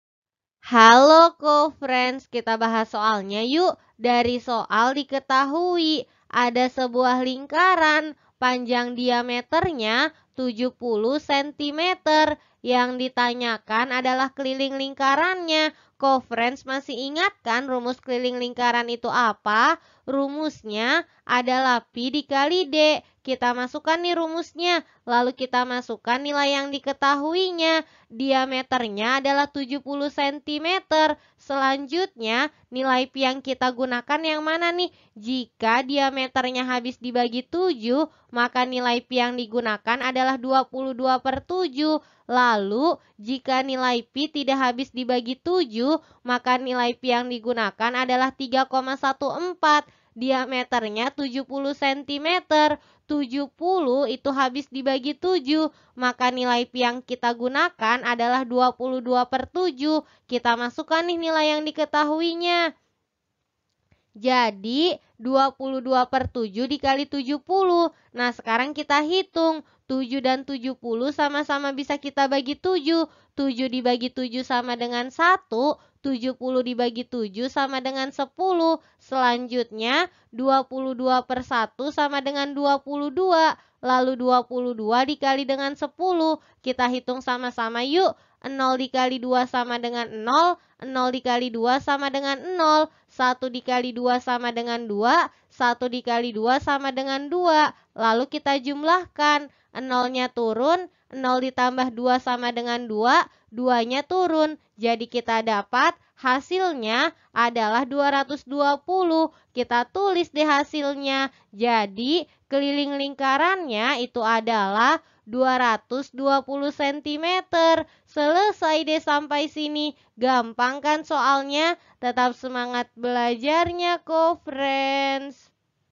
Keliling lingkaran yang memiliki panjang diameter 70 cm adalah ....
Topik atau Materi: Keliling Lingkaran - Lingkaran - Geometri - Matematika Kelas 6 SD - K-13 Revisi

Sumber Soal: MBP Matematika Kelas 6, K-13 Revisi, TMBP, 2021 , Masmedia Buana Pustaka

00:00 : Bedah Soal
01:00 : Konsep, rumus dan pengertian Keliling Lingkaran
01:18 : Langkah penyelesaian soal
01:05 : Jawaban akhir
01:35 : Penutup